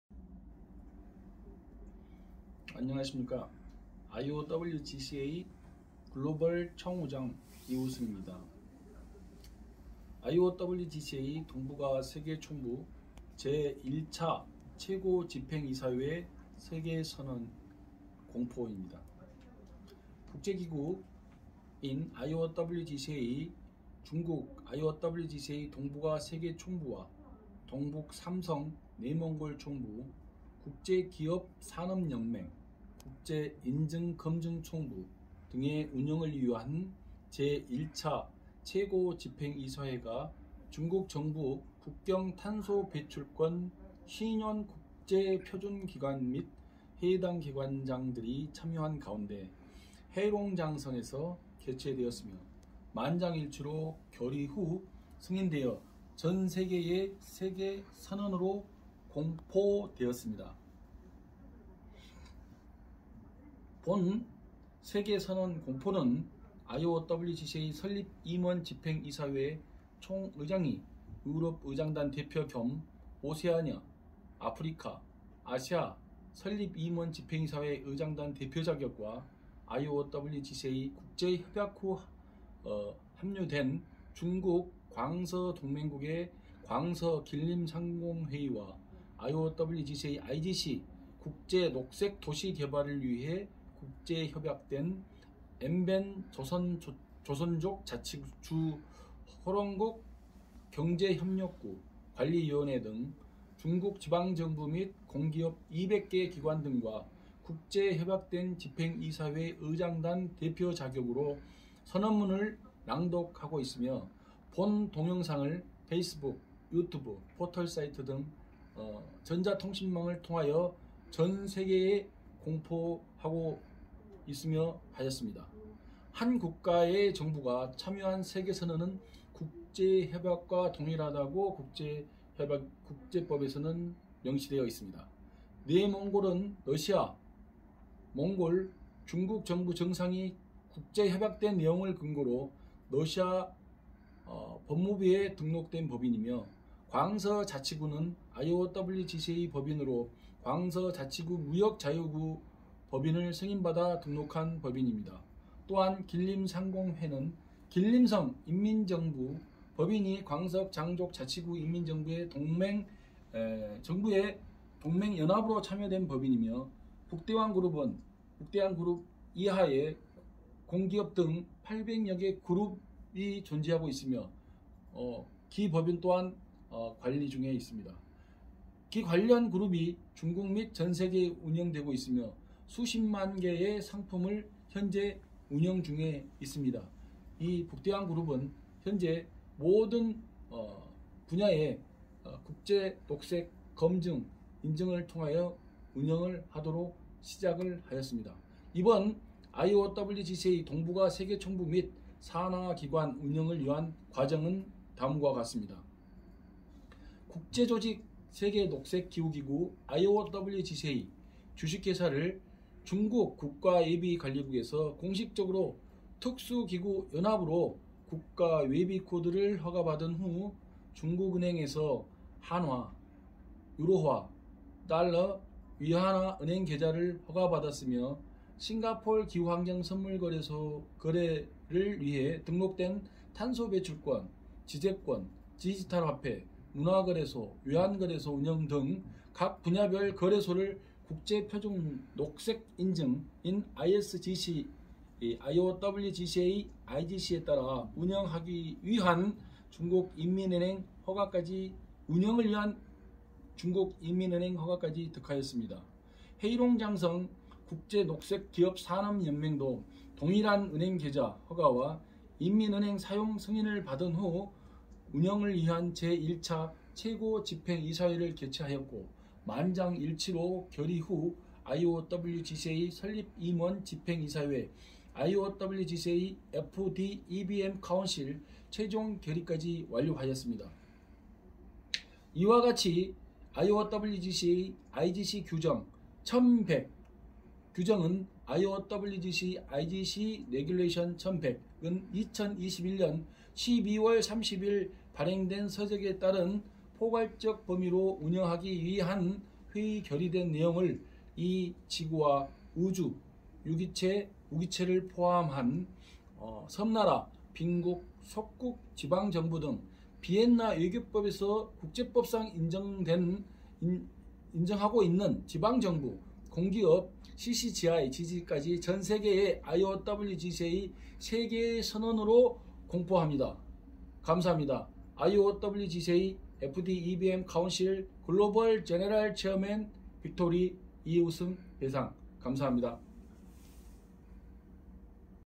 국제기구 세계녹색기후기구/IO(INGO국가가 아닌 국제기구)-WORLD GREEN CLIMATE ASSOCIATION/IO-WGCA东北亚（东盟）世界总部第一届最高理事会公布
Announced by the first supreme council of IO-WGCA Northeast Asia WHQ
International Organization World Green Climate Association Northeast Asia WHQ, IO-WGCA three northeast provinces (Inner Mongolia)  Harbin - verification and certification HQ, IO-WGCA International Green Enterprise Industry Union, international standard carbon sequestration and carbon emission Committee and other relevant institutions, Participated in the first Supreme Council meeting of IO-WGCA Northeast Asia WHQ held in Harbin, Heilongjiang Province. The meeting decided to announce to the world that "international standard green verification and certification, international standard enterprise industry union and carbon sink credit management have been fully launched". At the same time, the IO-WGCA F,D.EBM Council Global General Chairman serves as the representative of the European presidium and the presidium of the supreme executive councils of Oceania, Africa and Asia.
The institutions that have signed international agreements with IO-WGCA include more than 200 Chinese local governments and public welfare enterprises at all levels, such as China's Guangxi Jilin Chamber of Commerce and Industry and the Management Committee of Helong border economic cooperation zone of Yanbian Korean Autonomous Prefecture. The representative of the presidium of the IO-WGCA F,D.EBM Council delivered a congratulatory message and watched the live video, which has been released to the world through electronic communication networks such as YouTube and portal.
According to the contents of the international agreement, IO-WGCA is registered as a legal person in the free trade zones of Harbin and Guangxi respectively, and has signed an international agreement with Beidahuang food industry group to promote and operate more than 13000 kinds of green products around the world.
Projects already operated by IO-WGCA Northeast Asia WHQ and subsidiaries include: International Organization World Green Climate Association INC. is approved by Tianjin Branch of State Administration of foreign exchange of China to assign overseas codes to special institutions, and Bank of China authorizes the establishment of bank accounts using won, euro, US dollar and RMB, International remittance bank code (swift code) is registered with industrial and Commercial Bank of Heilongjiang Province.
According to the operation needs of international standard green verification and certification (isgc) "IO-WGCA IGC", the joint climate and environmental carbon emission and carbon sink exchange, intellectual property exchange, digital currency exchange, culture, foreign exchange and other exchanges are registered in Singapore.
The results of the first Supreme Council meeting of IO-WGCA International Green Enterprise Industry Union unanimously adopted the IO-WGCA F.D, EBM Council. Therefore, according to the contents of the conference resolution of the full range operation of IO-WGCA IGC regulation 1100 (IO-WGCA IGC regulation 1100: issued on December 30, 2021), island countries, poor countries, dependent countries, local governments, etc., including the earth, the universe, organisms and non organisms, public welfare enterprises CCGI and GGG are published to the world as the "IO-WGCA World Declaration".
Thanks!
IO-WGCA F,D.EBM COUNCIL GLOBAL GENERAL
CHAIRMAN VICTORY LEE.

IO-WGCA东北亚（东盟）世界总部第一届最高理事会公布
国际组织(INTERNATIONAL ORGANIZATION)--IO-WGCA(国际组织世界绿色气候机构)东北亚（东盟）世界总部、绿色气候国际验证认证东北三省（内蒙古）总部、国际绿色企业产业联盟世界总部、国际标准碳汇碳排放权委员会等相关机构负责人，参加了在黑龙江省哈尔滨市举办的东北亚（东盟）世界总部第一届最高理事会会议。会议决定向全世界公布“国际标准绿色验证认证、国际标准企业产业联盟及碳汇信用管理各项工作全面启动”。同时由IO-WGCA最高执行理事会总议长担任欧洲主席团代表，兼大洋洲、非洲、亚洲最高执行理事会主席团代表。
与IO-WGCA签订国际协议的有：“中国广西吉林工商会”和“吉林省延边朝鲜族自治州和龙边境经济合作区管理委员会”等中国各级地方政府及公益企业200多家机构。并由最高理事会主席团代表致贺辞，观看了现场视频，通过YouTube、门户网站等电子通信网络已向全世界公布。
根据国际协议内容在中国东北三省（内蒙古）哈尔滨和广西自由贸易区分别注册为法人，并与北大荒食品产业集团签订了国际协议，一万三千多种绿色产品在全球推广运营。
IO-WGCA东北亚（东盟）中国总部和下属机构已经运营的项目包括：国际组织世界绿色气候机构(IO-WGCA)股份公司被中国国家外汇管理局天津分局批准为特殊机构境外代码赋码、中国银行授权设立使用韩元、欧元、美元、人民币的银行账户，在黑龙江省工商银行注册了国际汇款银行编码（SWIFTCODE）。
按照国际标准绿色验证认证(ISGC)“IO-WGCA IGC”运营需要，在新加坡注册气候环境碳排放碳汇联合交易所，知识产权交易所、数字货币交易所、文化、外汇等各领域交易所。
国际绿色企业产业联盟世界总部第一次最高理事会会议决议，一致通过了设立成员最高执行理事会(IO-WGCA F.D EBM COUNCIL)。因此，根据IO-WGCA IGC条例1100(IO-WGCA IGC REGULATION 1100:2021年12月30日发行)的全范围运营的会议决议的内容，包括地球、宇宙、有机体和无机体在内的岛国、贫穷国、属国、地方政府等、公益企业CCGI，GGG作为“IO-WGCA世界宣言”向全世界公布。

谢谢！
CHAIRMAN VICTORY LEE.